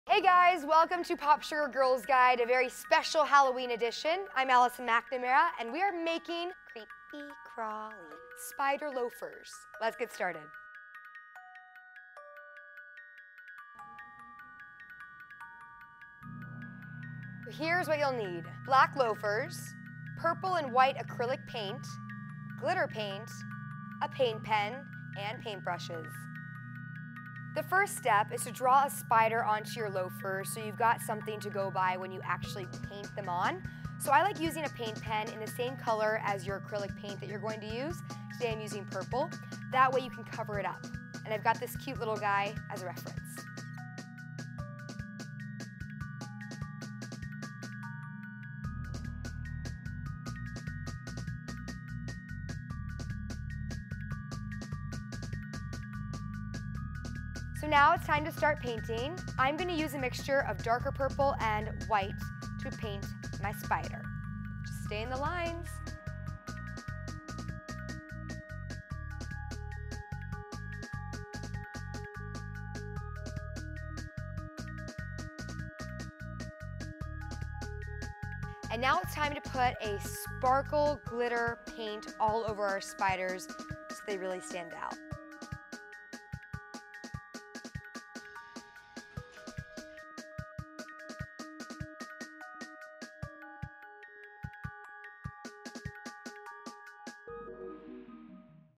How to Make DIY Spider Loafers For Halloween
We just got started on Vine!  Watch us there, too!